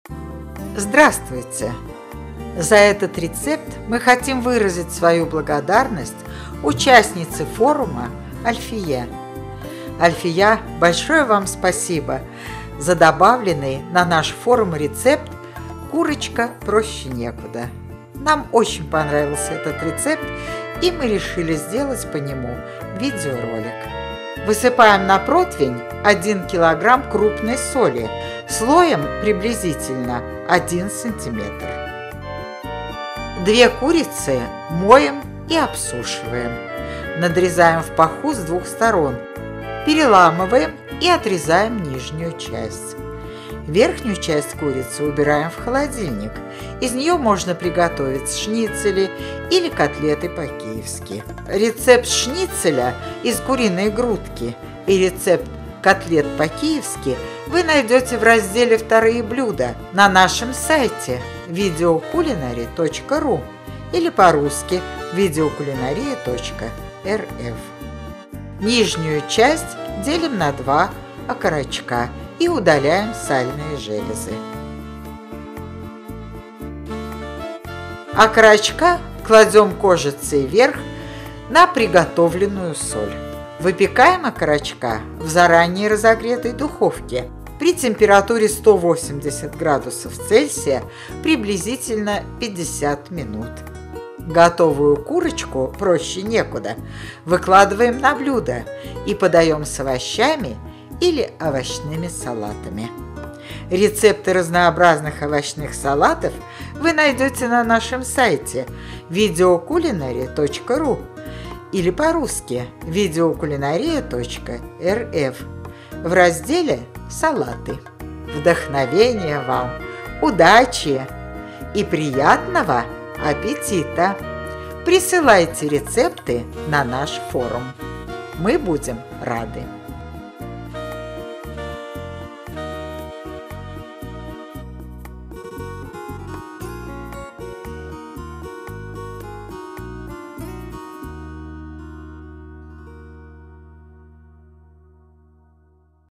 Курочка проще некуда - Рецепт Бабушки Эммы
Как приготовить Курочка проще некуда — рецепт и советы от Бабушки Эммы. За этот рецепт мы хотим выразить свою благодарность участнице форума Альфие. Альфия, спасибо Вам за добавленный на наш форум рецепт Курочка проще некуда. Нам очень понравился этот рецепт, и мы решили сделать по нему видеоролик. Куриные окорочка, приготовленные по этому рецепту, действительно, получились очень вкусные, несмотря на простоту приготовления. Курочка проще некуда — рецепт с фото и видео. Бабушка Эмма делится Видеорецептом Курочки проще некуда - посмотреть подробный пошаговый рецепт и задать вопросы
Ингредиенты:
Куриные окорочка – 4 штуки
Соль крупная – 1 килограмм
Приблизительная пищевая ценность на 100 грамм продукта:
Калорийность - 157.87
Белки - 16.79
Жиры - 10.19
Углеводы - 0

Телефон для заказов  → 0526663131
Во многих наших видео рецептах мы используем музыку композитора Даниила Бурштейна